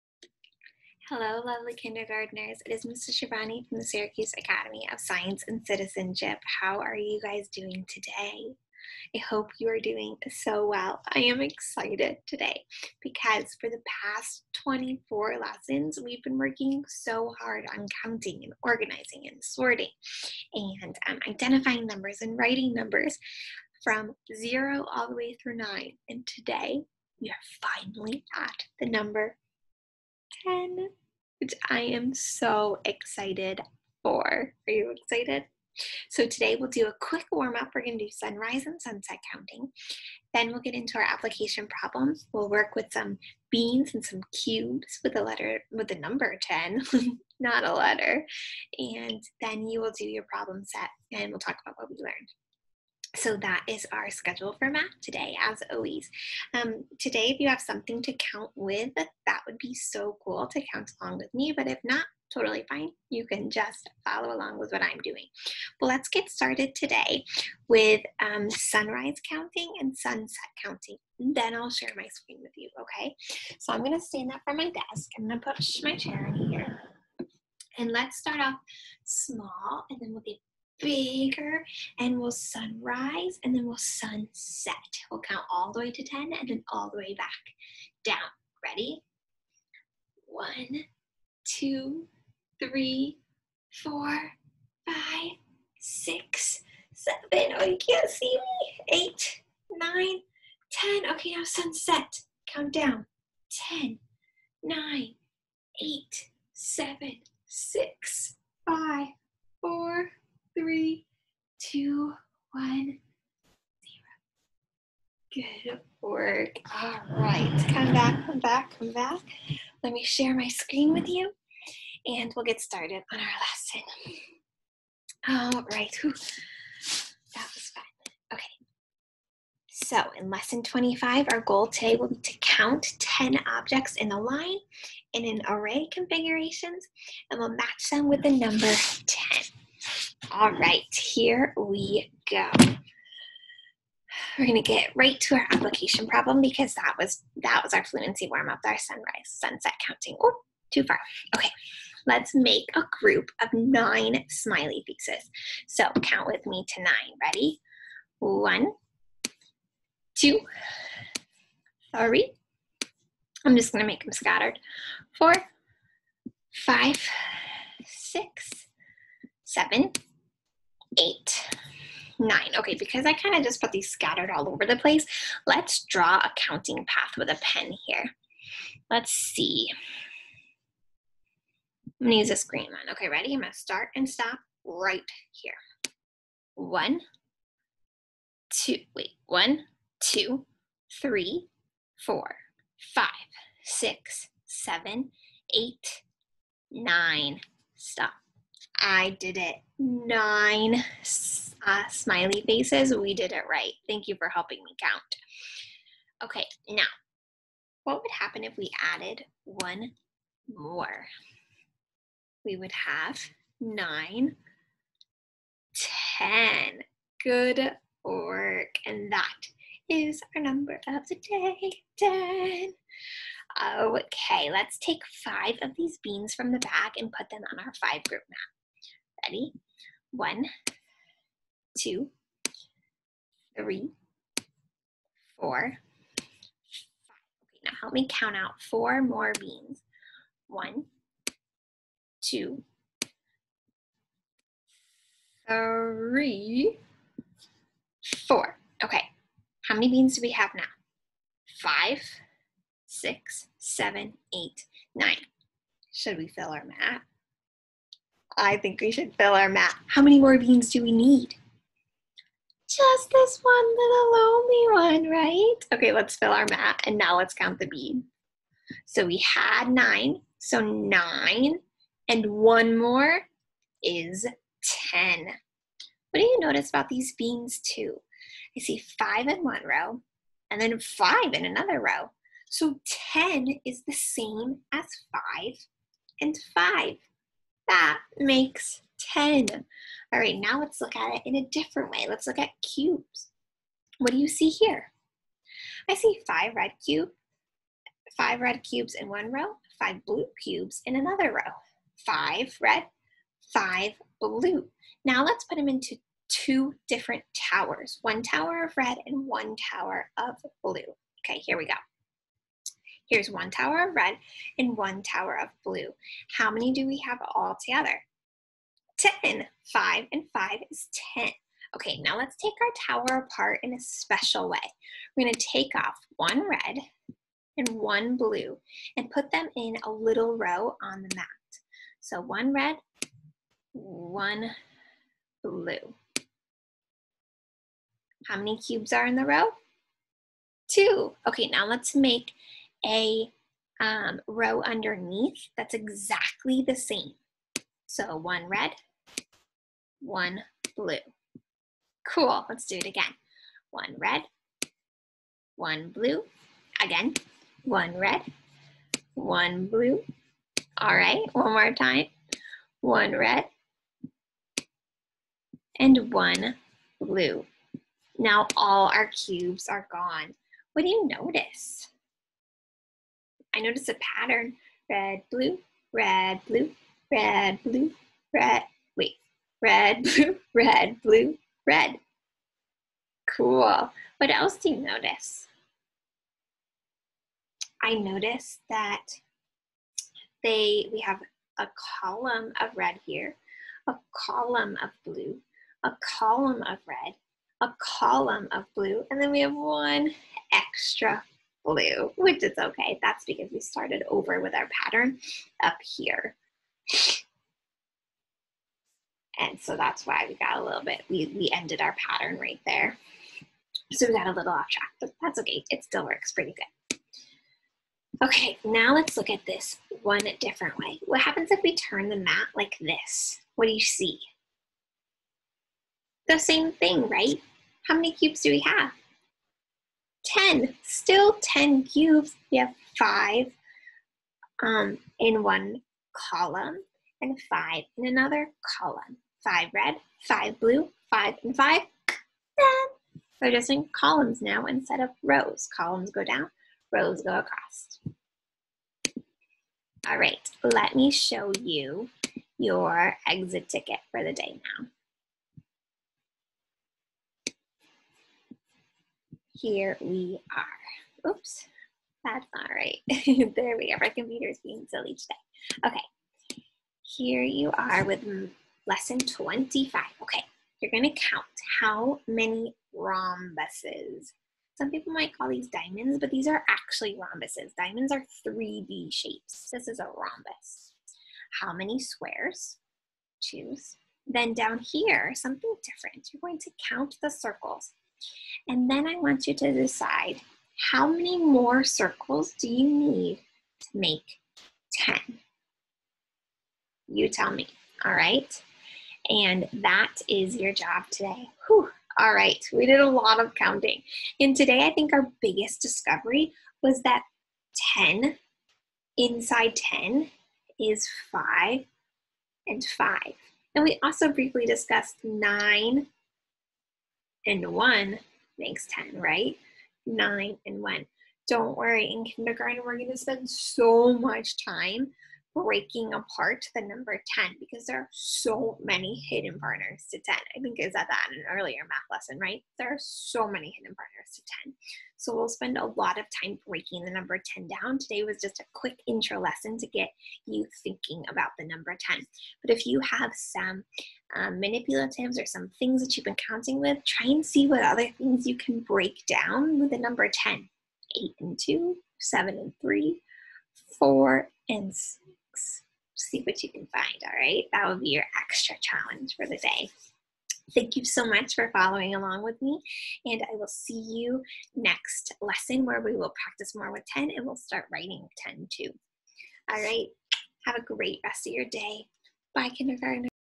Engage NY: Kindergarten: Module 1: Lesson 25
Objective: SWBAT count 10 objects in linear and array configurations (5 and 5), match with numeral 10, place on the 5-group dot mat. SWBAT dialogue about 9 and 10 on the mat and write numeral 10.